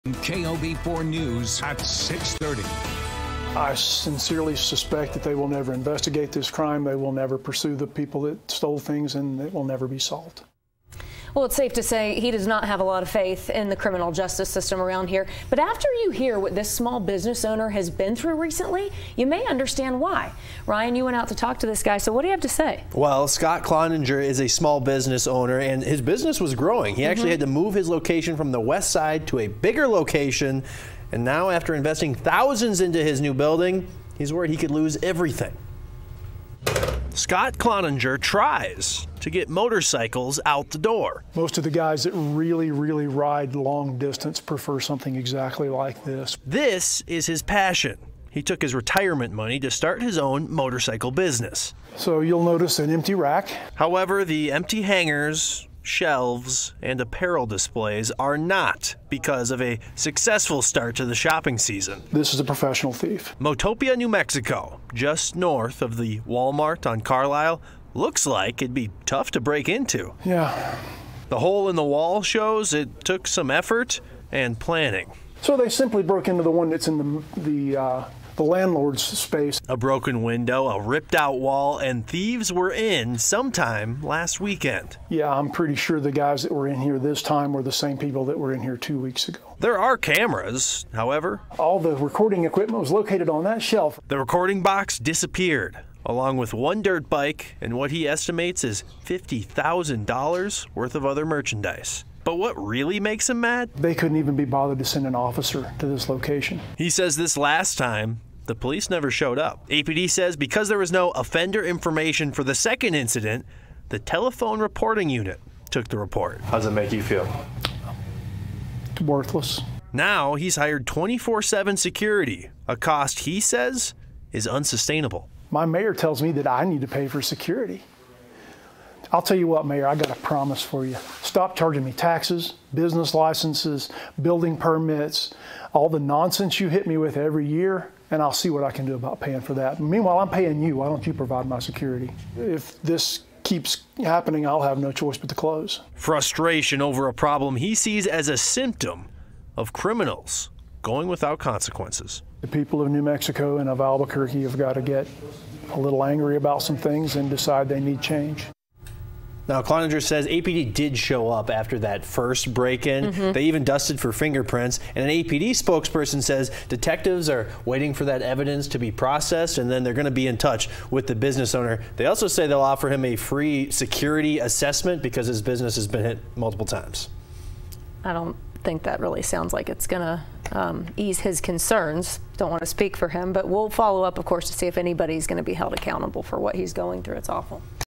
Small Albuquerque business owner frustrated after multiple robberies
After investing thousands into a new building, one small business owner in Albuquerque is worried he could lose everything. Story